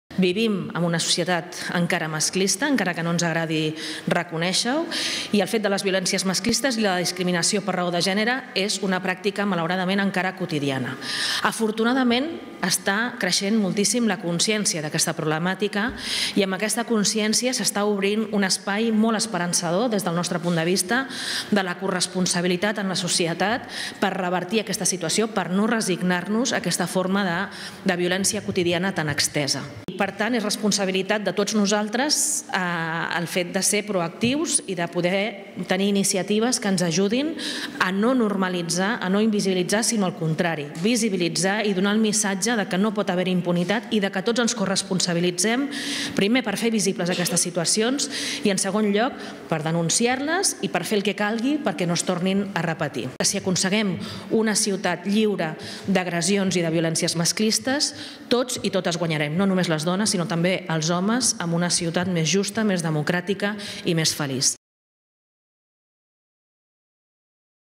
Ada Colau sobre el protocol "No Callem"
Presentem un nou protocol conjunt amb una desena de sales de concerts i festivals de música de la ciutat per actuar contra les agressions i els assetjaments sexuals en espais d’oci nocturn.